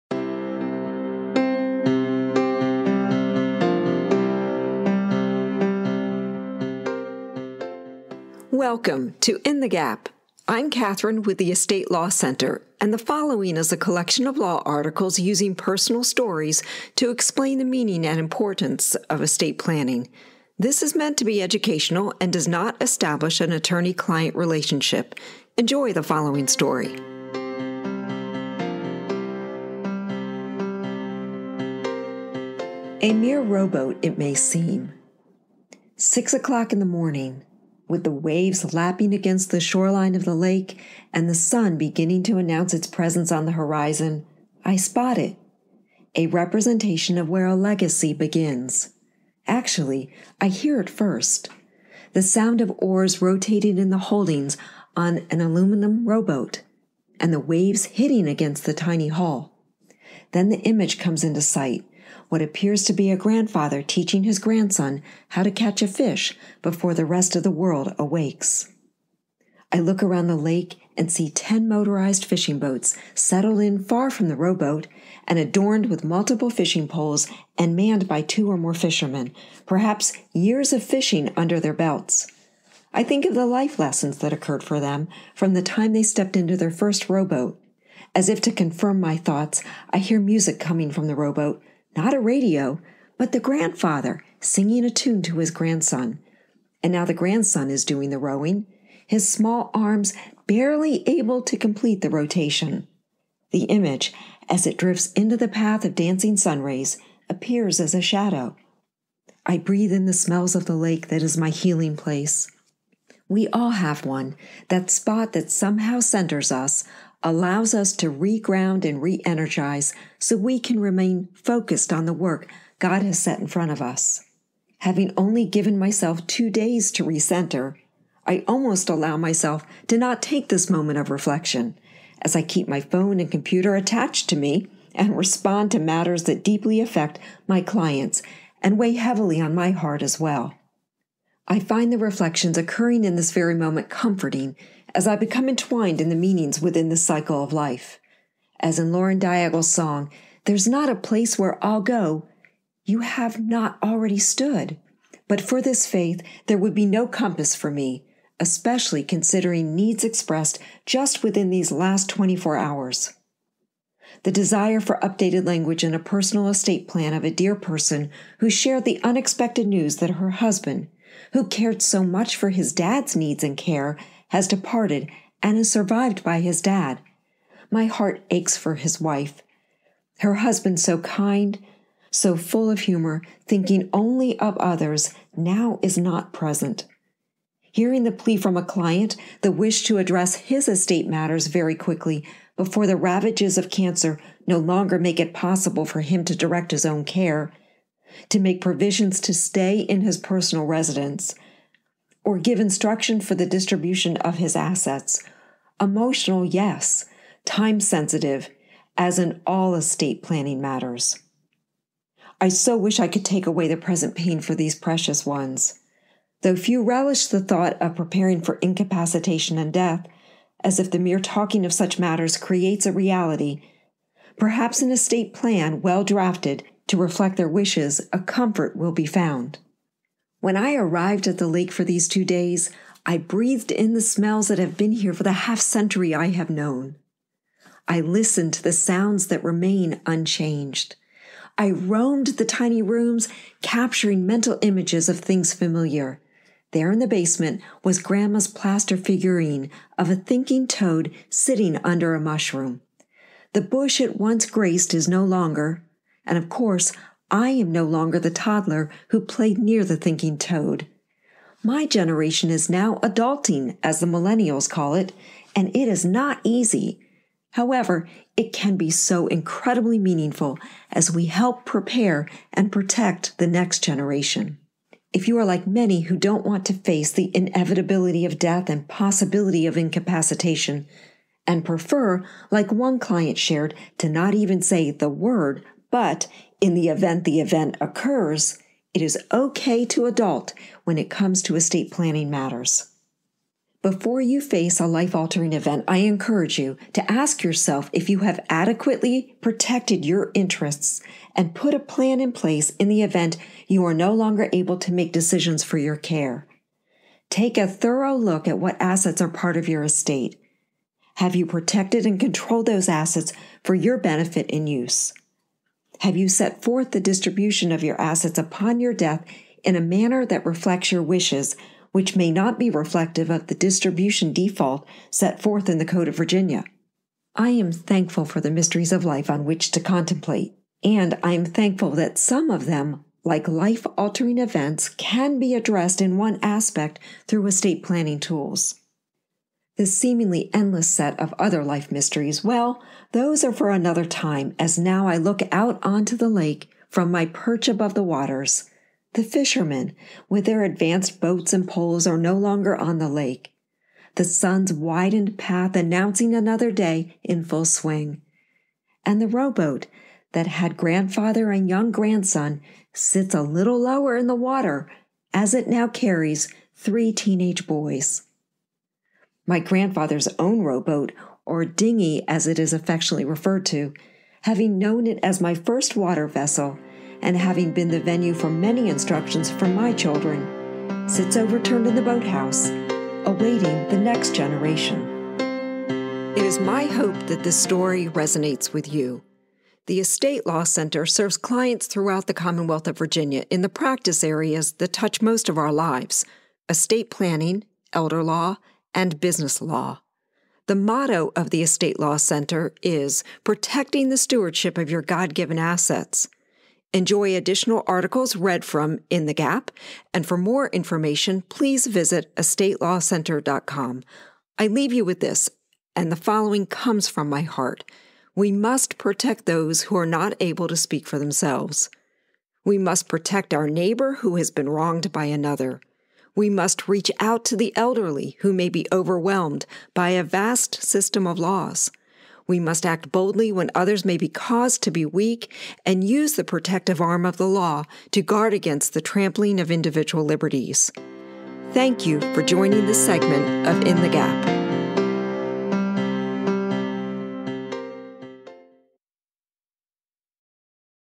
Asset Protection & Life Mysteries
Estate Law Center Podcast Episode #21 - Stories in Estate Planning in the Commonwealth of Virginia.